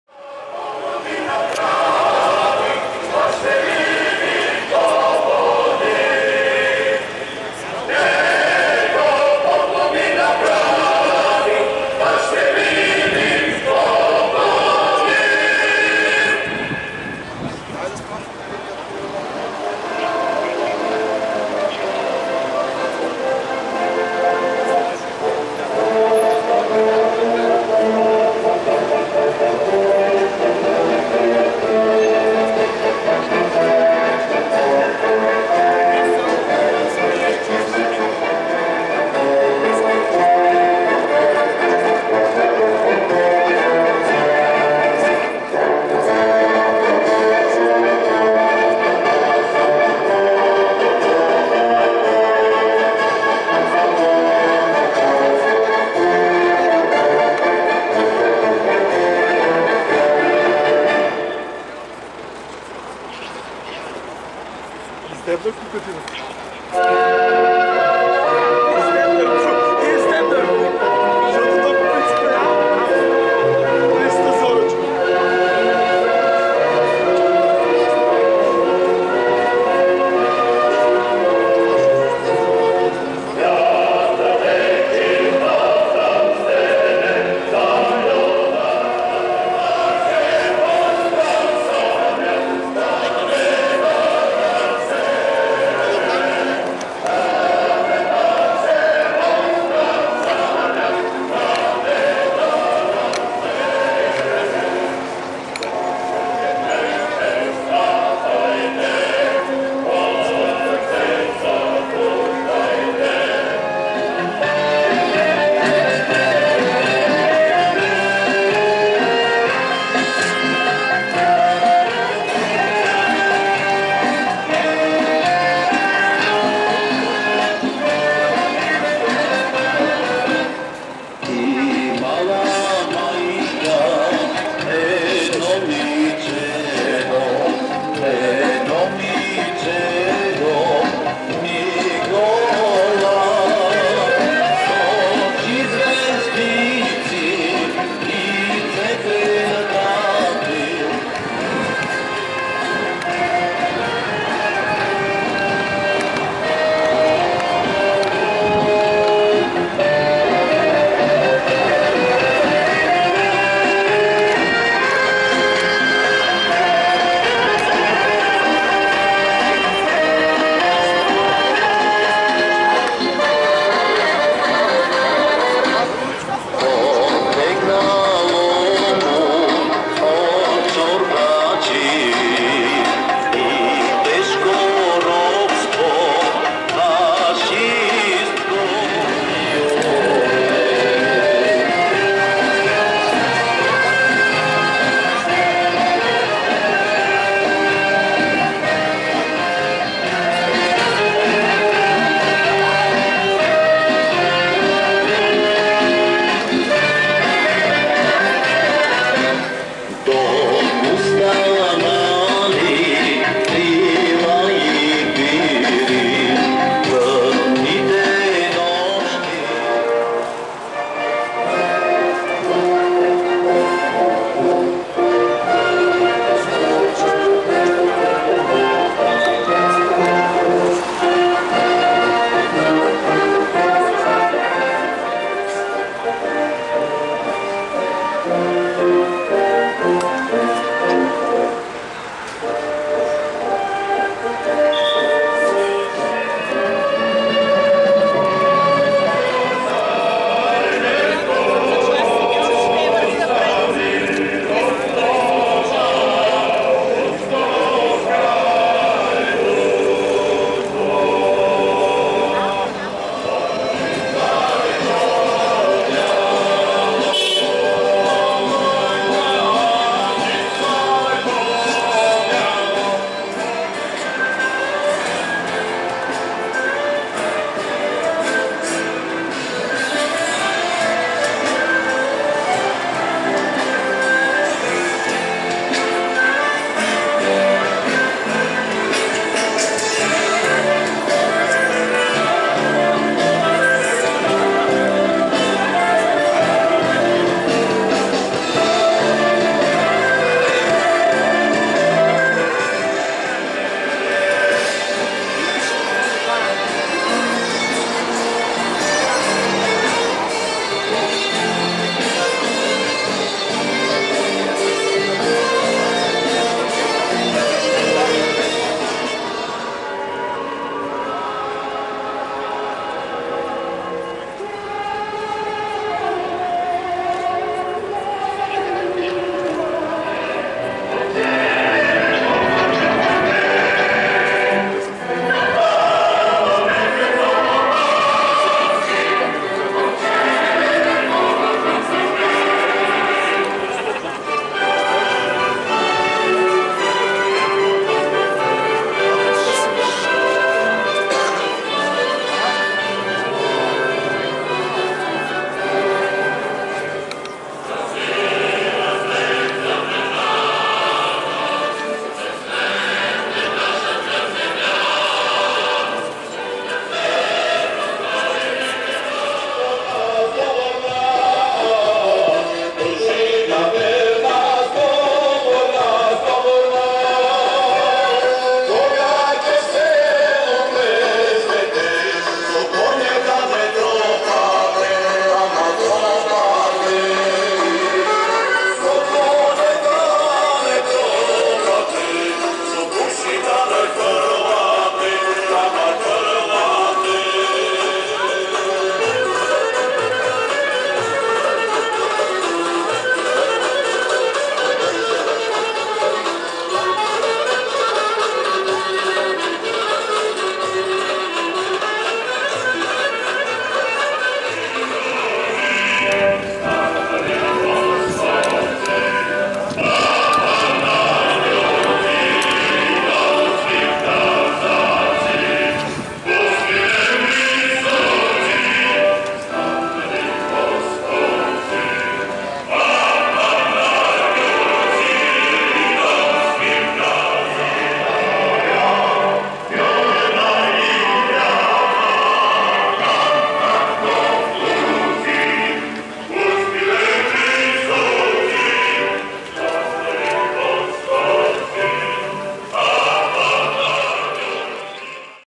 Шествие за 3 март в София
По случай празника много граждани взеха участие в  шествието по бул Цар Освободител и ул  Раковска